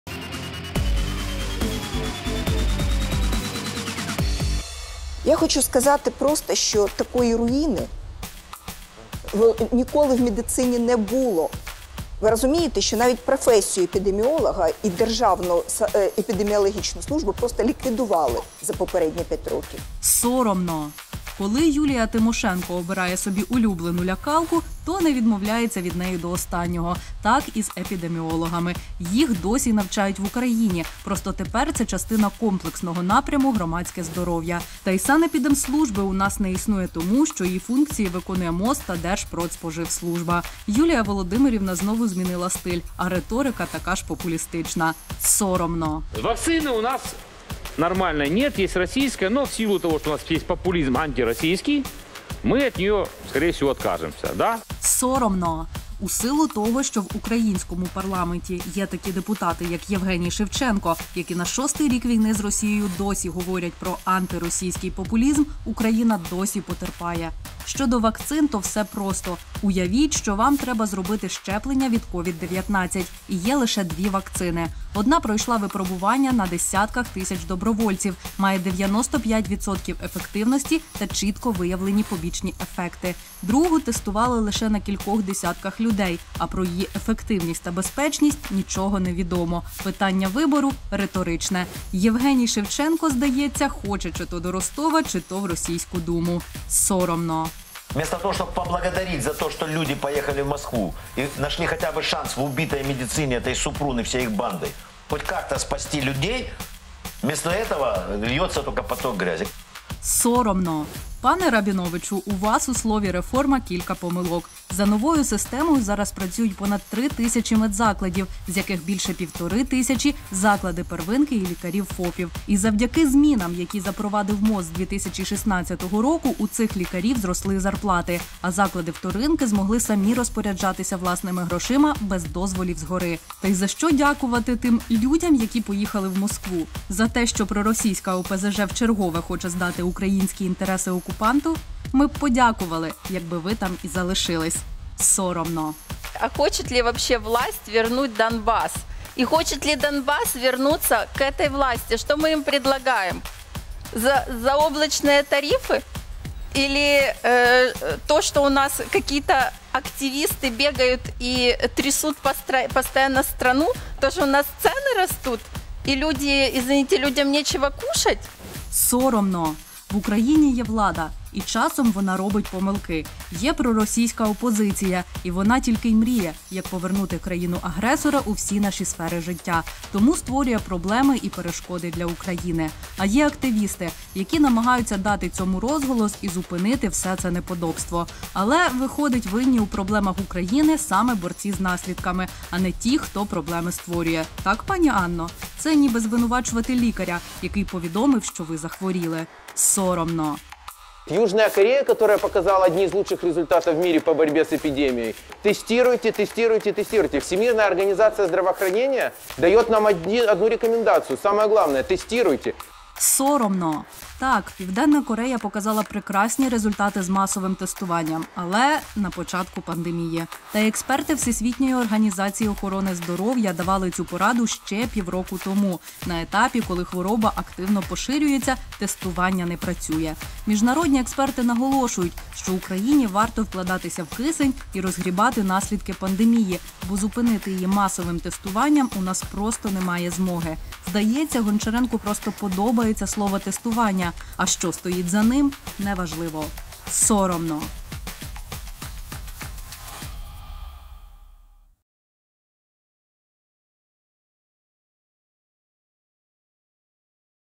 VoxCheck & Соромно. Тимошенко, Шевченко, Рабінович, Скороход, Гончаренко
Юлія Тимошенко, депутатка, лідерка ВО «Батьківщина»:
«Я хочу сказати, що просто такої руїни в медицині ніколи не було. Ви розумієте, що навіть професію епідеміолога і державну санепідеміологічну службу просто ліквідували за попередні п’ять років».

Євгеній Шевченко, депутат, фракція «Слуга Народу»:
«Вакцини (від коронавірусу – ред._ у нас нормальної немає. Є російська, але зважаючи на те, що у нас є популізм антиросійський, ми, скоріш за все, відмовимось».

Вадим Рабінович, депутат, фракція «Опозиційна платформа – За життя»:
«Замість того, щоб бути вдячними, що люди поїхали в Москву і знайшли хоча б шанс в знищеній медицині цієї Супрун і всією їхньою бандою хоч якость врятувати людей – замість цього ллється тільки потік бруду».

Анна Скороход, депутатка, депутатська група «За майбутнє»:
«І чи хоче Донбас повернутися до цієї влади? Що ми їм пропонуємо? Захмарні тарифи? Чи те, що у нас якісь активісти бігають і трясуть постійно країну? Те, що у нас ціни ростуть і людям, вибачте, їсти немає чого?»

Олексій Гончаренко, депутат, фракція «Європейська Солідарність»:
«Південна Корея, яка показала один із кращих результатів в світі в боротьбі з епідемією, тестує, тестує і тестує! І ВООЗ дає нам одну рекомендацію: найголовніше – тестуйте».